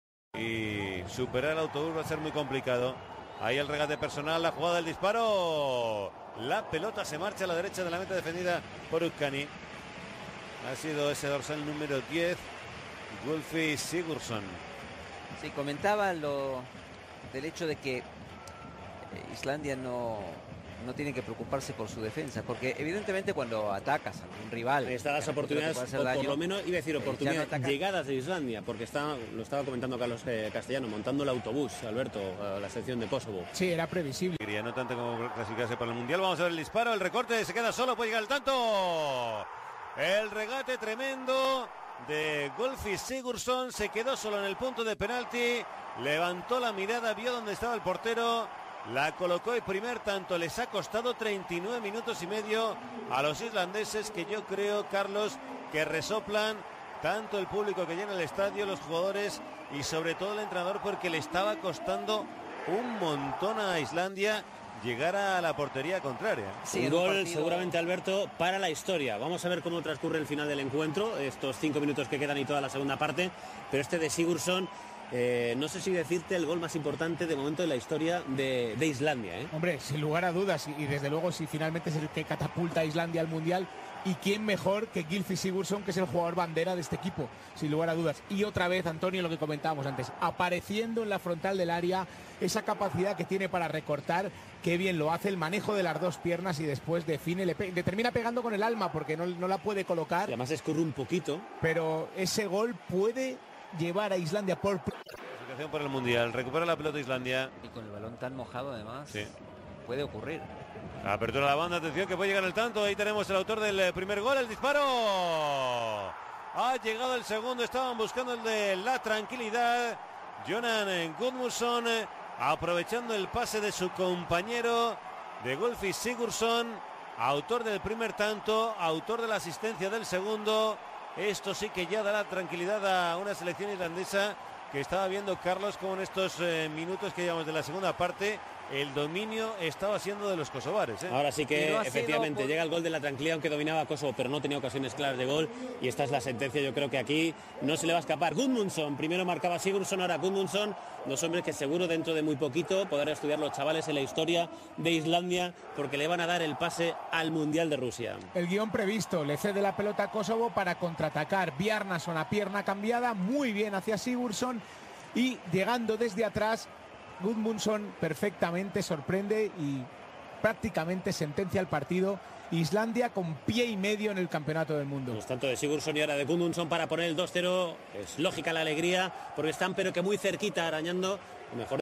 Iceland vs Kosovo 2-0 All Goals & Highlights 09/10/ 2017 {HD}
CONGRATULATION TO ICELAND  RUSSIA 2018
GOOD LUCK TO KOSOVO